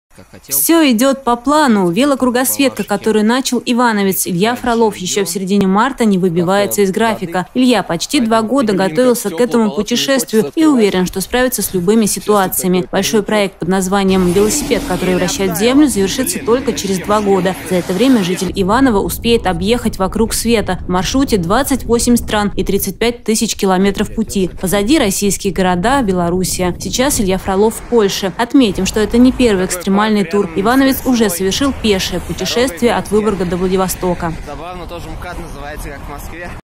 Велокругосветка идет по плану!
Все идет по плану. Велокругосветка, которую начал ивановец Илья Фролов еще в середине марта, не выбивается из графика. Илья почти два года готовился к этому путешествию и уверен, что справится с любыми ситуациями. Большой проект под названием «Велосипед, который вращает землю» завершится только через два года. За это время житель Иванова успеет объехать вокруг света. В маршруте 28 стран и 35 тысяч километров пути. Позади российские города, Белоруссия. Сейчас Илья Фролов в Польше. Отметим, что это не первый экстремальный тур. Ивановец уже совершил пешее путешествие от Выборга до Владивостока.